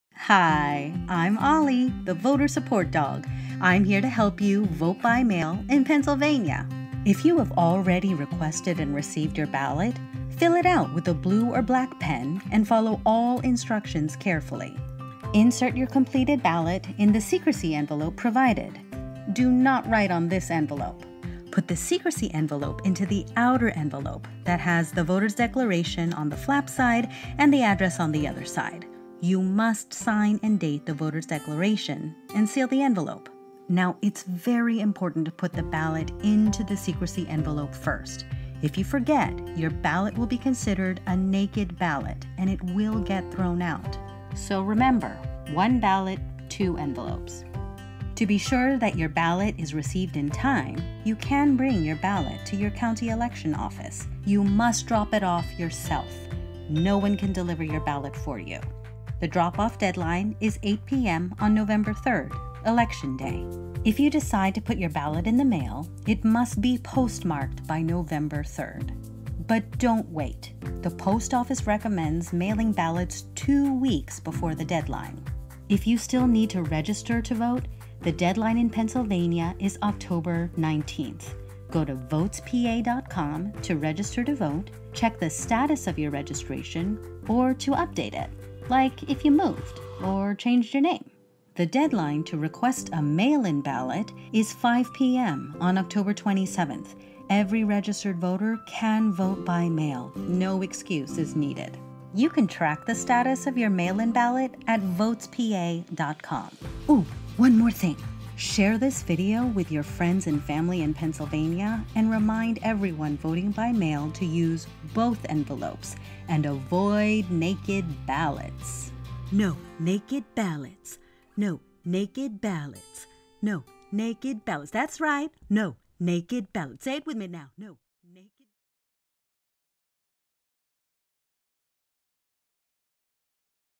ا يوجد بطاقات اقتراع فارغة في بنسلفانيا (No Naked Ballots in PA!)
لا "بطاقة اقتراع عارية"
اذهب الي الرابط التالي
لمزيد من المعلومات حول كيفية التصويت في بنسلفينيا

لتتعلم كيف يمكن انقاذ امريكا مع اصدقائك وحيواناتك الاليفة

[No Naked Ballots in PA! A Message from Ollie the Voter Support dog

Go to VotesPA.com for more information on how to vote in Pennsylvania.
Visit CountMeInToWin.org to learn how you can save America with your friends and pets.]